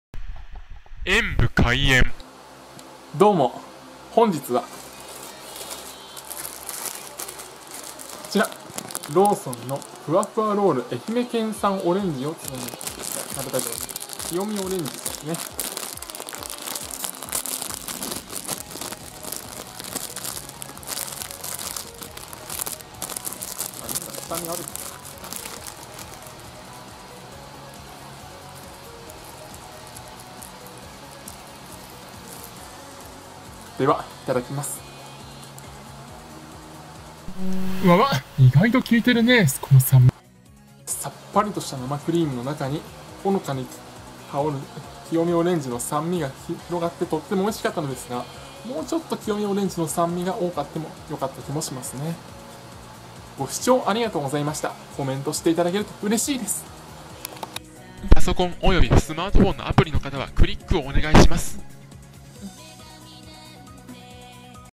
ローソン フワフワロール愛媛県産清美オレンジ使用を食べてみた オレンジの存在感凄い
ローソン　ふわふわロール愛媛県産清美オレンジ使用を食べてみました。オレンジの酸味がふわっと生地と共に来ると～